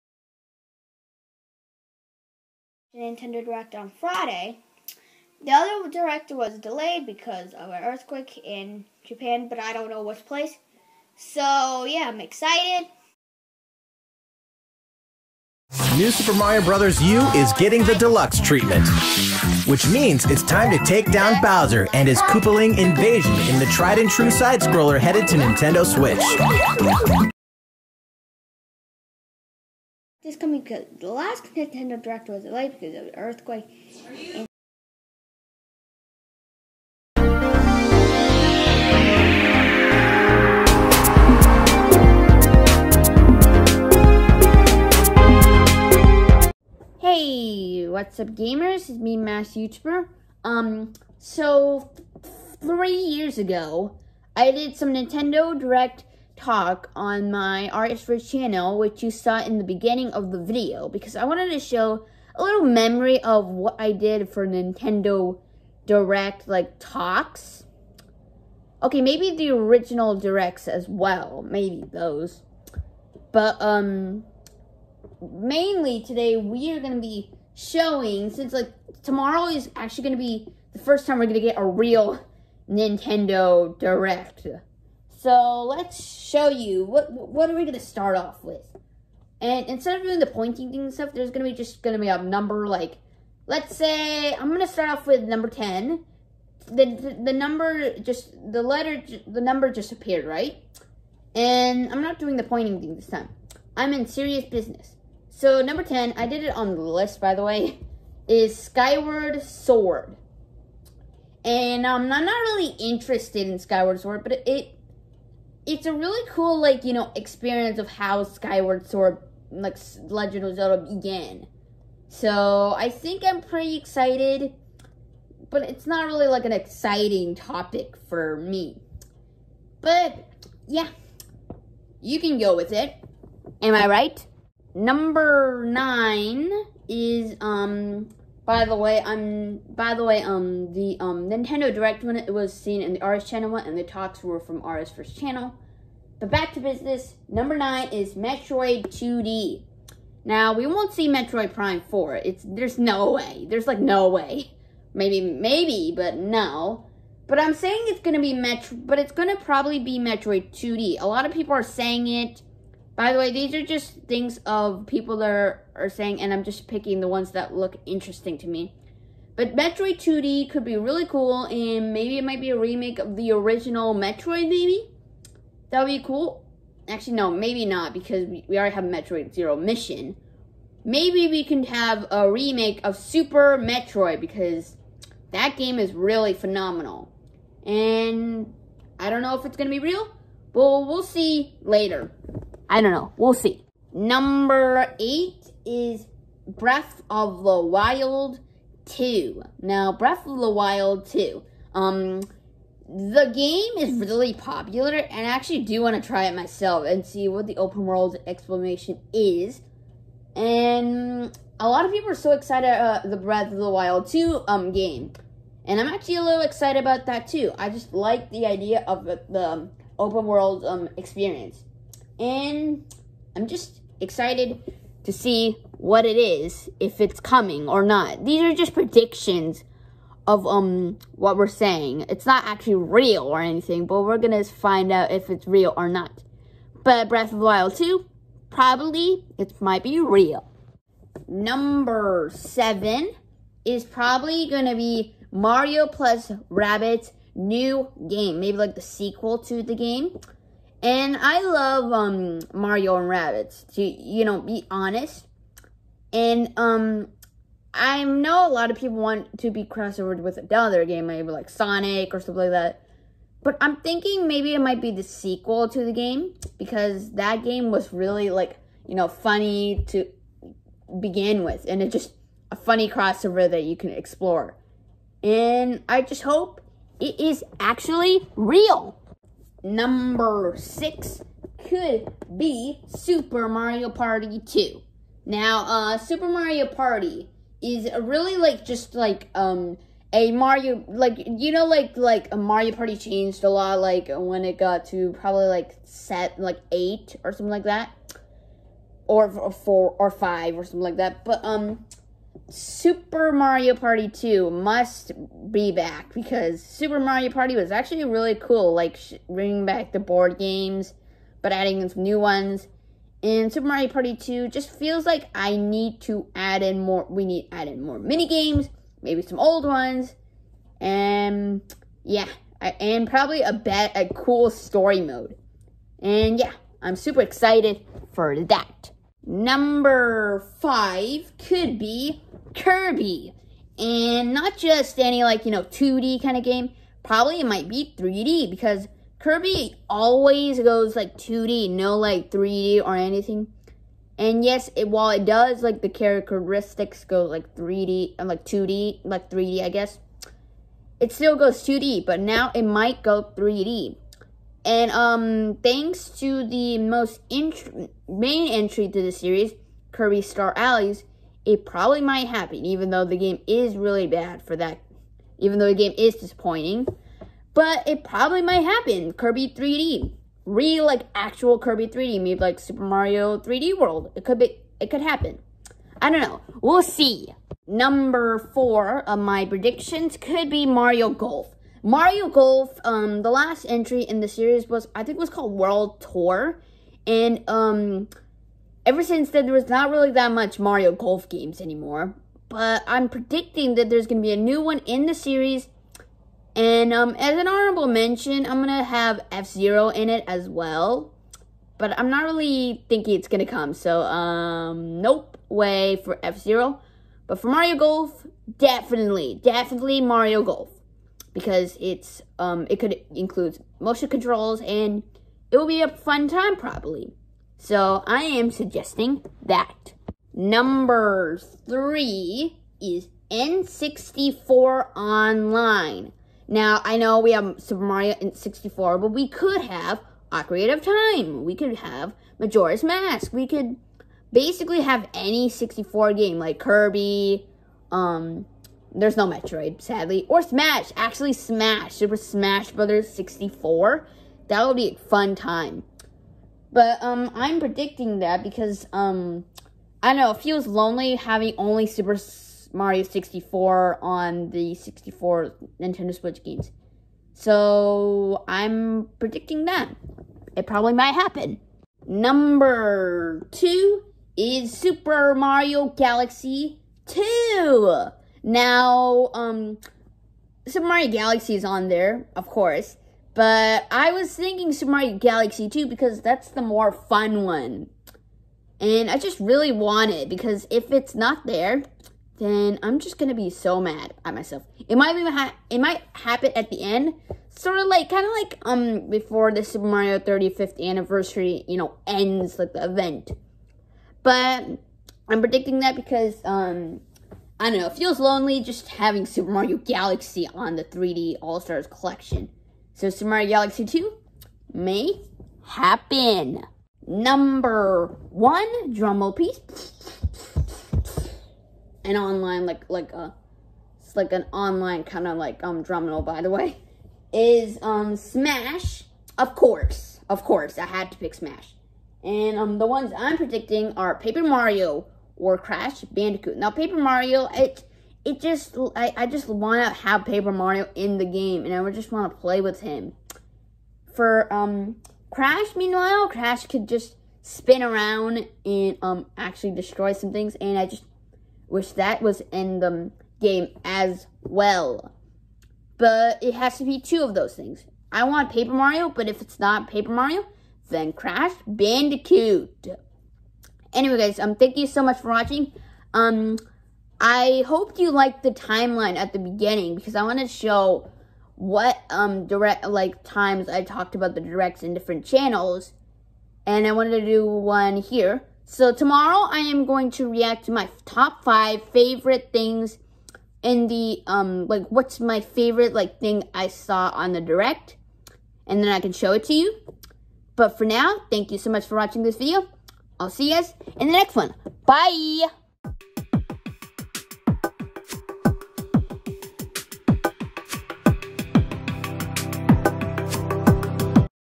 Top 10 Of My Favorite Nintendo Direct February 2021 Predictions!
I did a timeline of the direct videos I made in the past and came and did a prediction video for this new direct and show my top 10 of my favorite predictions!